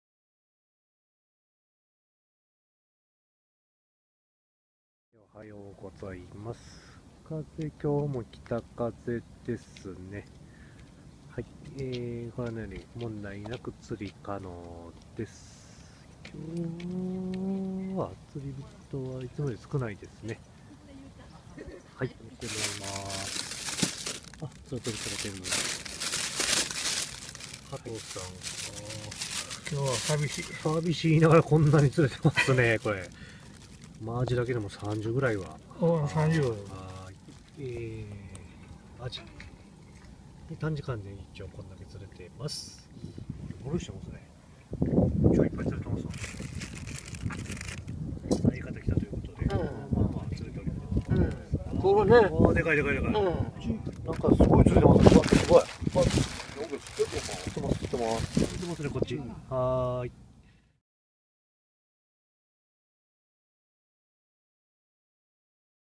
fishingmax芦屋 全体的に渋いです
アジの勢いがいつもほどありません。
サヨリちゃんは東向きでスタートです。